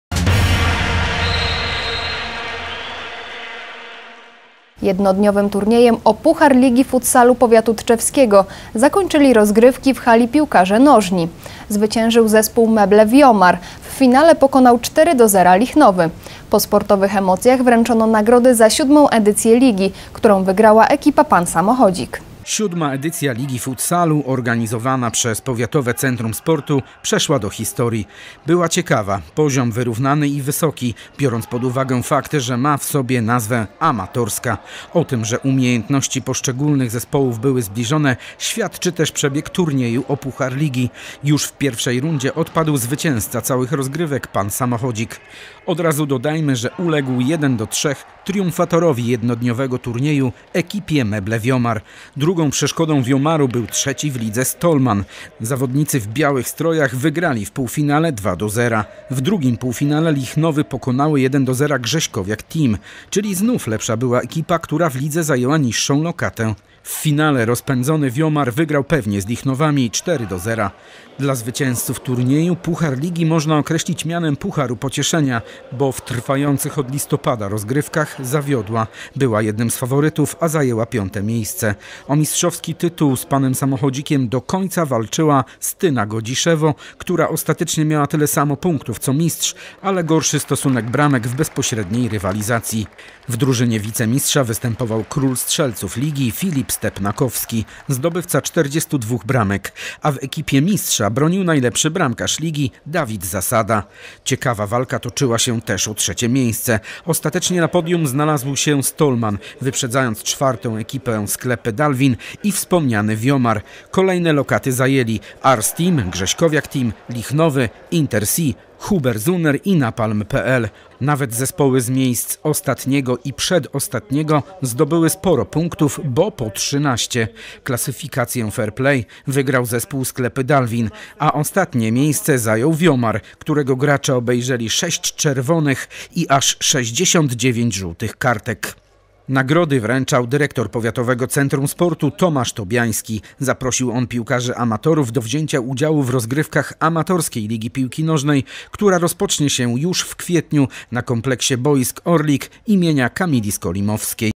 Wierzyca nie miała litości dla Aniołów - pewne zwycięstwo lidera - Tv Tetka Tczew HD
Wierzyca Pelplin wygrała 4:2 z Aniołami Garczegorze, dzięki czemu umocniła się na pozycji lidera pomorskiej IV ligi. Centrum Pelplin pauzowało, ale także wzbogaciło się o trzy punkty przyznane za walkower.